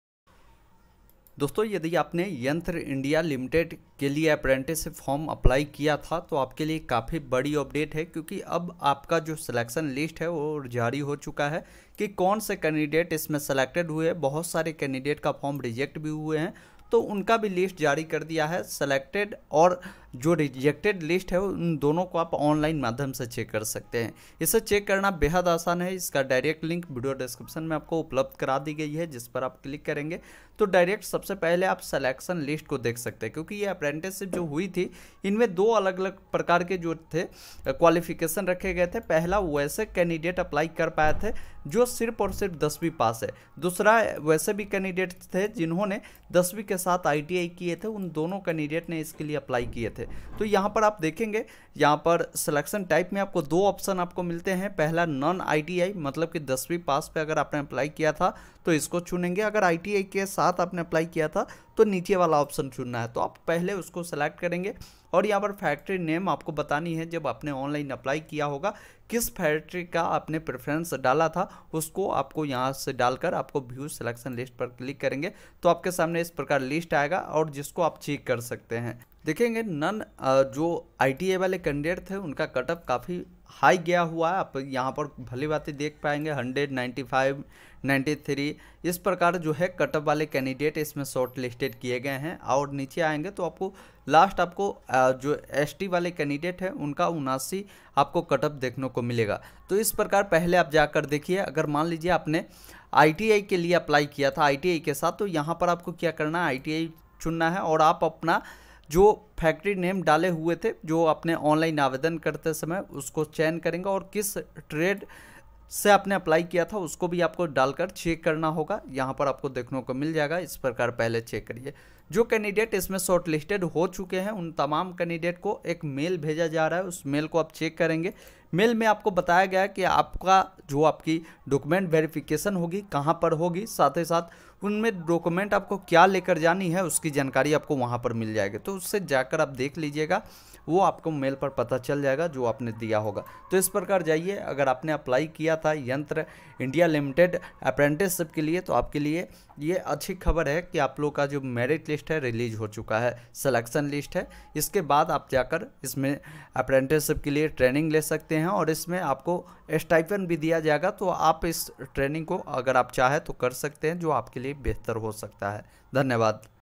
Yantra India Apprentice Merit List 2023 Kaise Download Kare
Hi
Dosto Aaj Ki Is Video Me Maine Aapko Ye Bataya Hai Yantra India Apprentice Merit List 2023 Kaise Download Kare | Yantra India Merit List 2023 Out  Ummeed Hai Aapko Ye Video Bahut Pasand Aayegi

Important Vidoes-